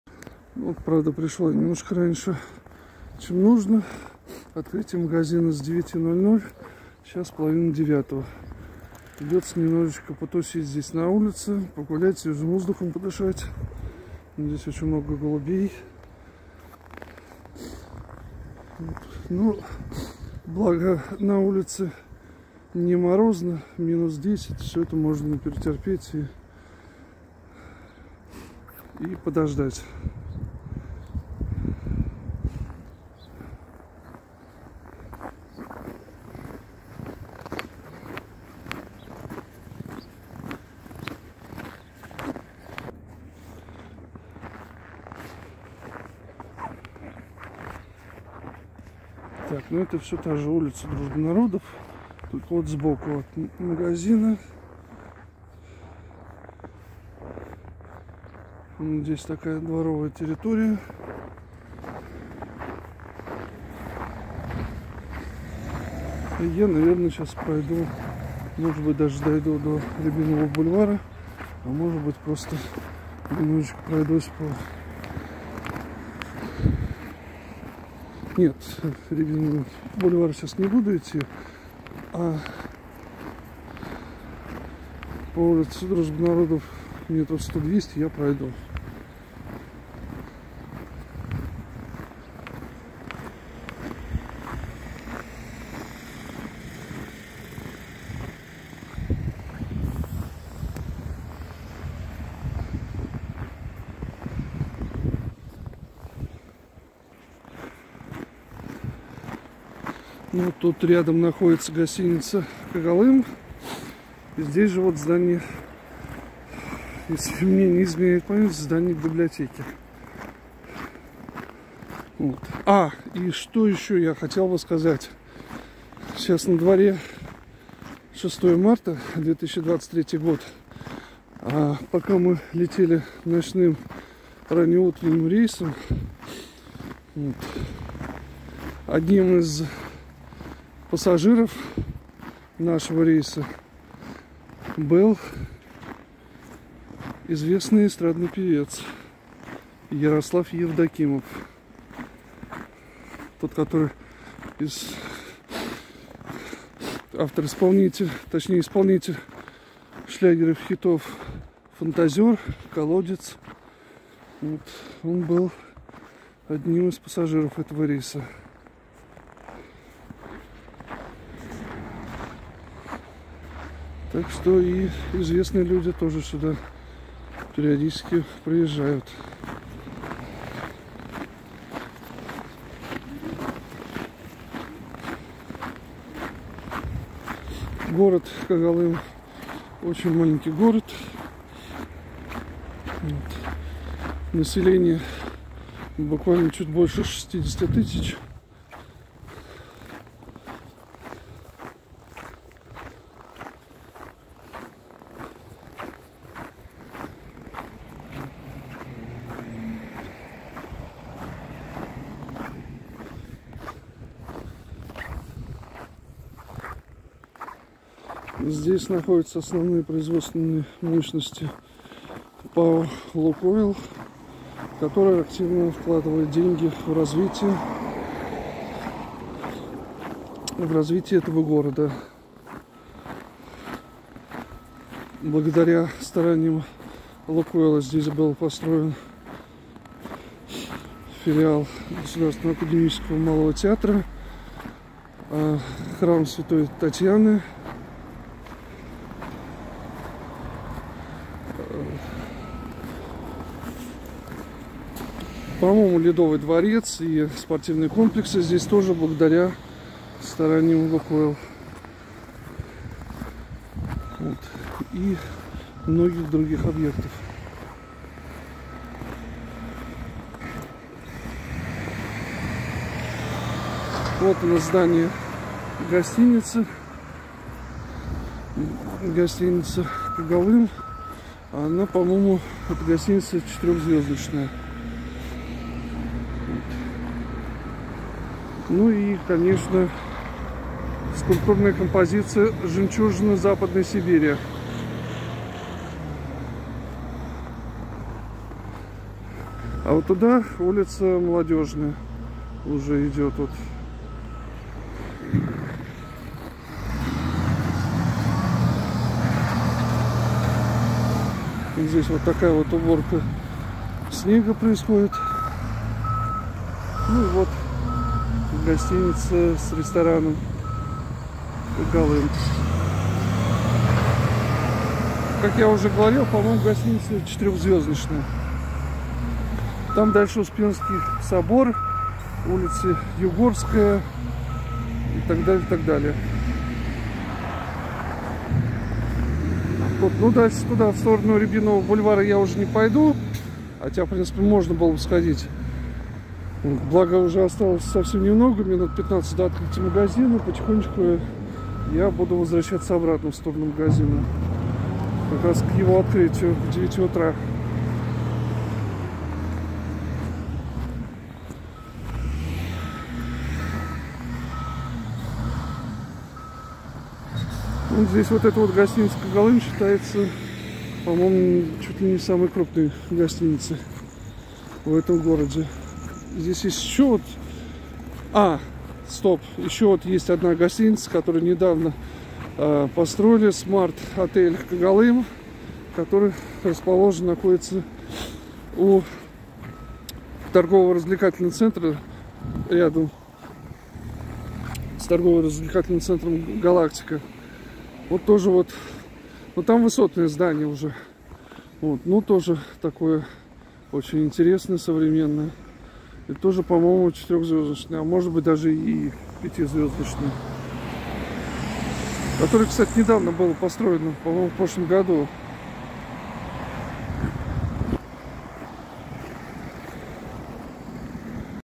Город Когалым. Улица Дружбы Народов. У гостиницы Когалым. Kogalym City. Март 2023
В ожидании открытия магазина Магнит в 9:00 по местному времени, я решил прогуляться по улице Дружбы Народов до гостиницы Когалым.
Март 2023